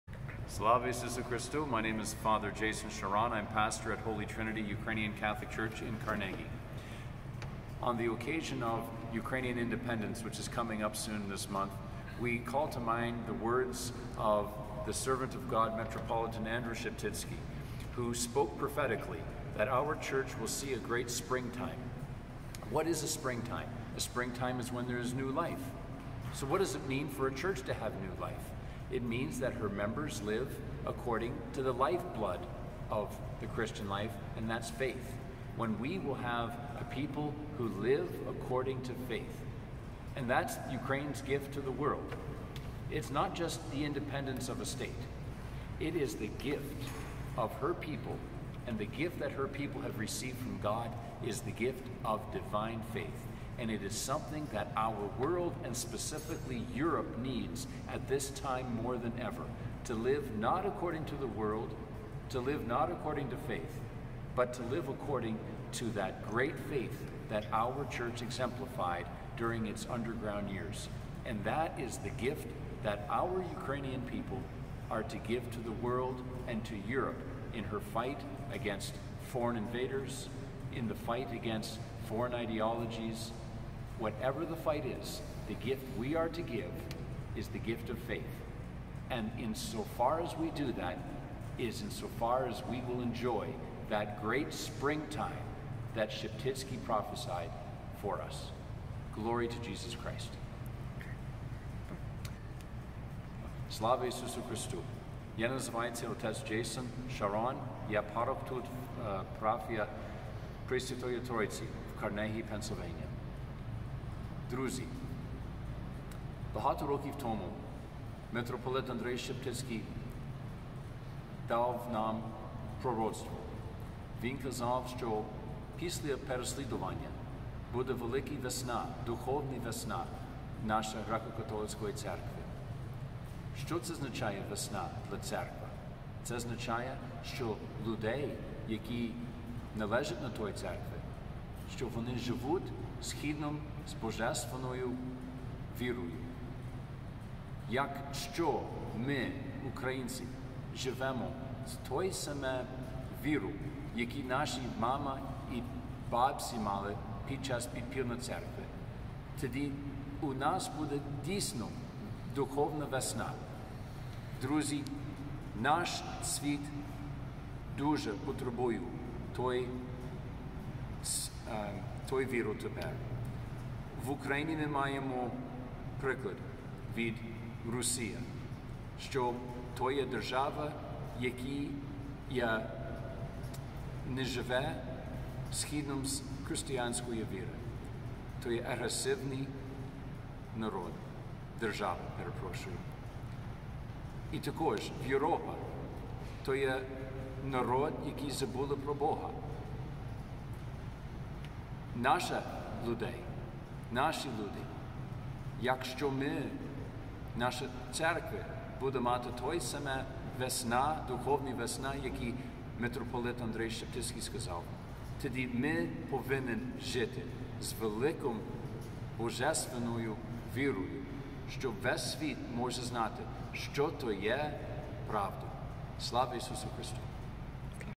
Вебінар радіопрограми Христос посеред нас "
Fr.Jason Charron
Holy Trinity Ukrainian Catholic  Church. Вебінар - вшанування  155 річниці  від дня народження Митрополита Андрея Шептицького.